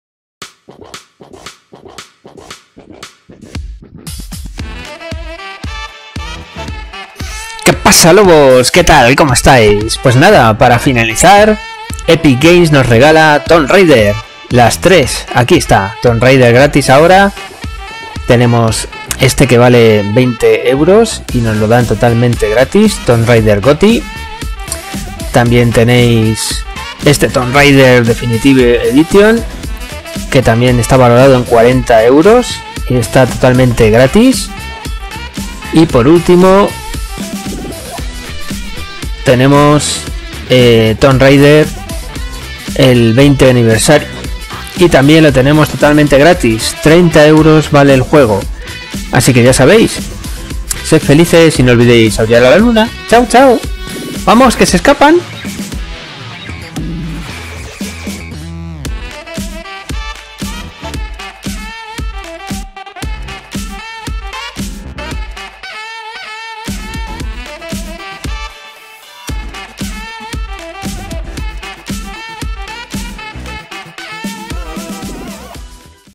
👉 Tomb Raider Trilogy "GRATIS" en Epic Games Store PC 🤑
Epic Games Store nos ofrece el último juego gratuito como parte de su promoción navideña en 2021. Los elegidos son la recopilación de los Tomb Raider modernos.
Tomb Raider Trilogy es el elegido de esta semana. Las aventuras de Lara Croft, desarrollado por Crystal Dynamics, se encontrará sin coste adicional hasta el 6 de enero a las 17:00 (CET).
Cuando completes el canjeo quedará ligado a tu perfil en el cliente para siempre. Debemos recordaros de que se trata de la versión completa, es decir, no encontrarás restricción de ningún tipo de contenido.